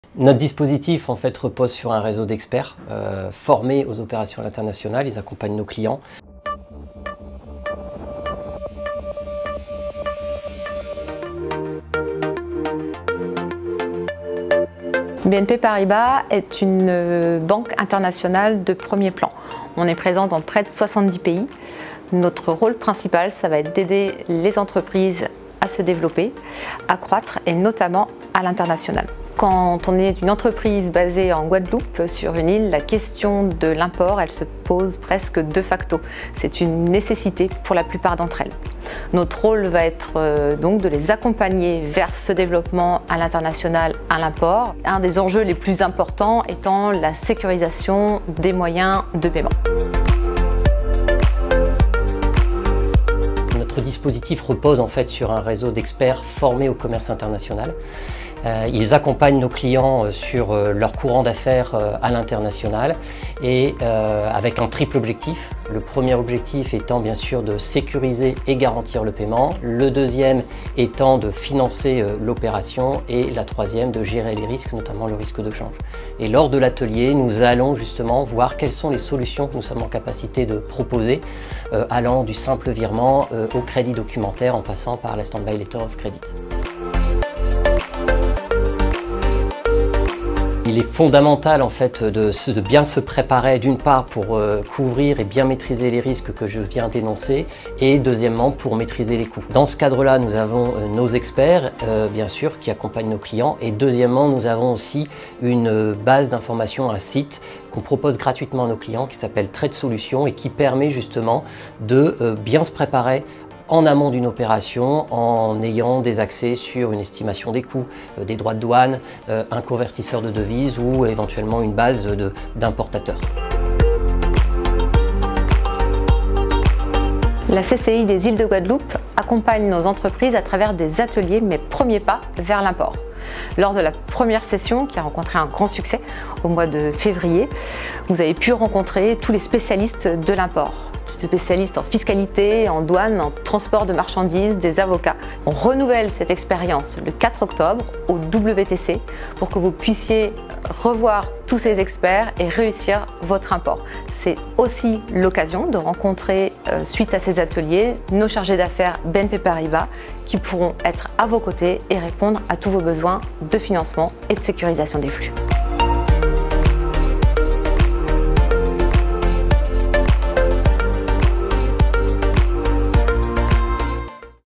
Mes Premiers pas à l'import - PITCH
Le 04 octobre 2022 aura lieu un atelier sur le thème des premier pas à l'import organisé par la CCI des Îles de Guadeloupe, avec, notamment, la présence d'experts de BNP Paribas qui  pourront répondre aux questions sur les besoins de financement et la sécurisation des flux.